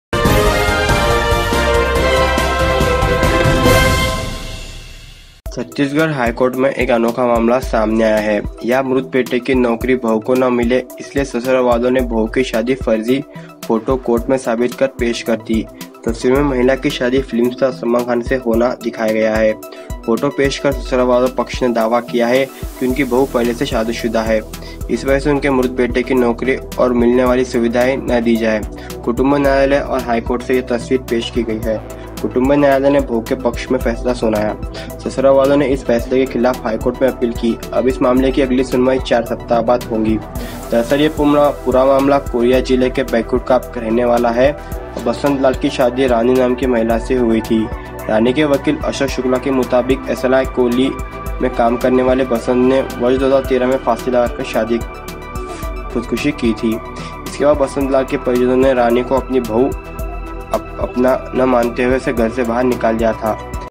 महिला ने सलमान खान से शादी होने का किया दावा, सबूत में पेश कर दिया फ़ोटो! फिर जो हुआ..!
Woman claims to be married to Salman Khan, photo presented in evidence! Then what happened ..!

A unique case has come up in the Chhattisgarh High Court. Here, the daughter-in-law does not get the job of the dead son, so the in-laws presented fake photos of the daughter-in-law's wedding as evidence in the court. In the picture, the woman is shown to be married to film star Salman Khan. By presenting the photo, the in-laws side claimed that their daughter-in-law is already married.
Because of this, he should not be given the job and facilities of the deceased son. This picture has also been presented in Kutumb Court and High Court. The family court ruled in favor of the daughter-in-law. The well-off have appealed against this decision in the High Court. Now the next hearing of this case will be after 4 weeks.

Know what is the whole matter
Actually, this whole matter is related to Basantlal living in Baikunthpur in Korea district. Basantlal was married to a woman named Rani. According to Rani's lawyer Ashok Shukla, Basantlal, who worked in SECL colliery, committed suicide by hanging himself in the year 2013. After this, the family members of Basantlal did not consider the queen as their daughter-in-law and drove her out of the house. It is being told that Rani's in-laws did not want her to get any benefit after the compassionate appointment and death of Basantlal. The matter reached the Kutumb court of Baikunthpur.

In-laws presented fake photos
In the Kutumb court of Baikunthpur, Rani's in-laws refused to accept her as his daughter-in-law. Also said that she is married to film actor Salman Khan. For this, he also presented a photo of Rani and Salman Khan in the court as proof. The court has ruled in favor of Rani, reprimanding her in-laws in the case. The in-laws have now appealed in the High Court against the order of the Kutumb court. In the preliminary hearing of this case, a single bench of Justice Gautam Bhaduri of the High Court has issued a notice to Rani. The next hearing of this case will be after 4 weeks.